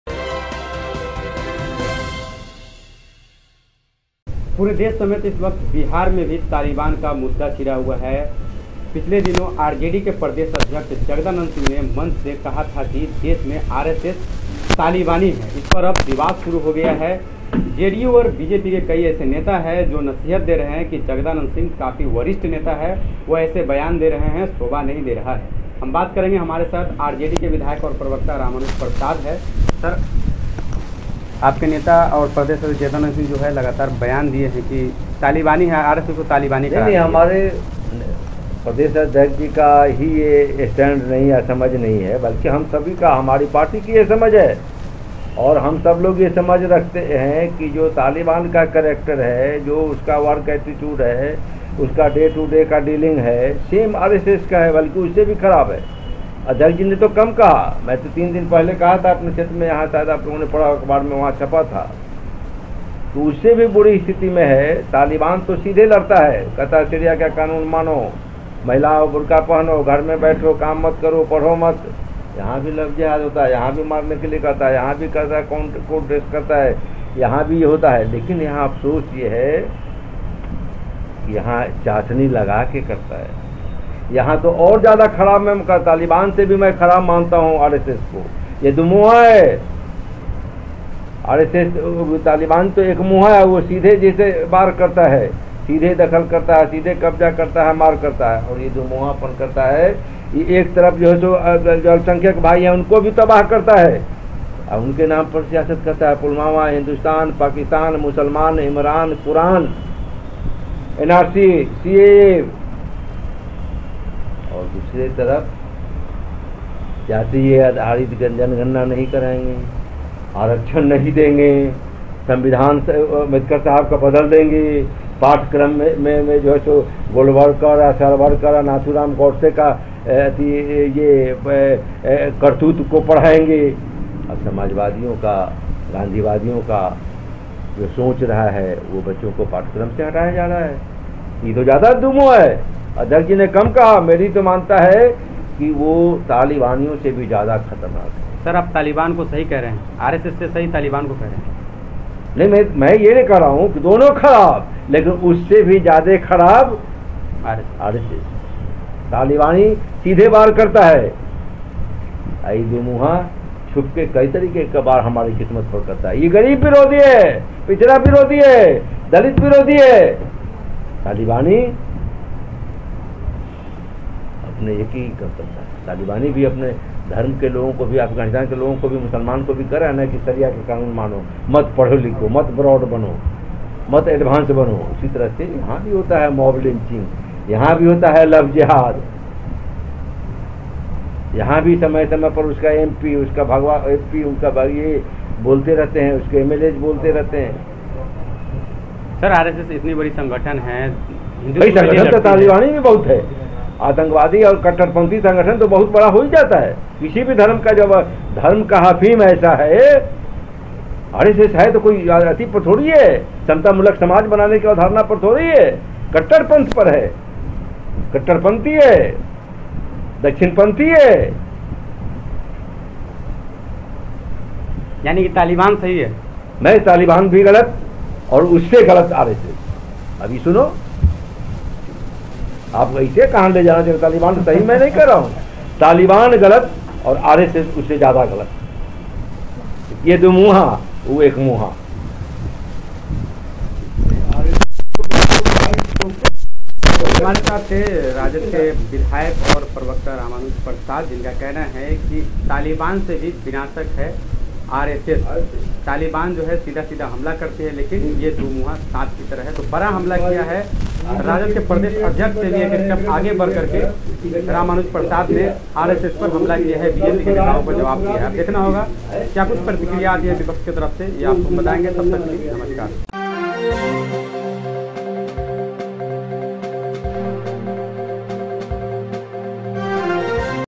RJD के प्रदेश अध्यक्ष Jagdanad के साथ आ गयी पूरी RJD | RSS और Taliban में बताया क्या-क्या है समानता
RJD के प्रदेश अध्यक्ष Jagdanad Singh के साथ आ गयी पूरी RJD | RSS और Taliban में बताया क्या-क्या है समानता